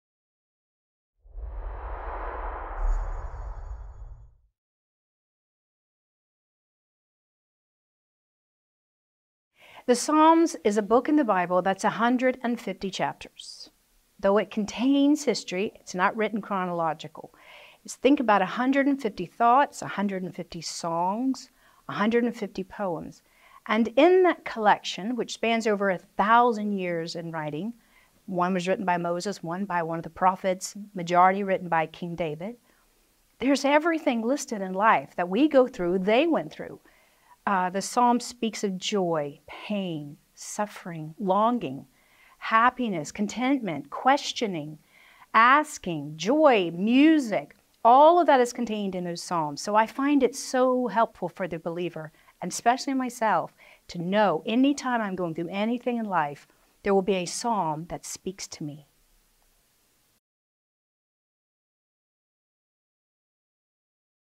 How do the Psalms speak into different areas of human life? - Angela MacKenzie
Angela Courte MacKenzie is a broadcaster, pianist, vocalist, and worship leader. Angela has a B.A. degree in music/vocal performance from the University of Central Florida, and more recently an M.A. in apologetics from Talbot School of Theology, Biola University. Here she answers questions related to her talk "How Do We Live out the Most Important Command the Scripture Gives: Loving God?," given at the 2022 European Leadership Forum.

22INT29A2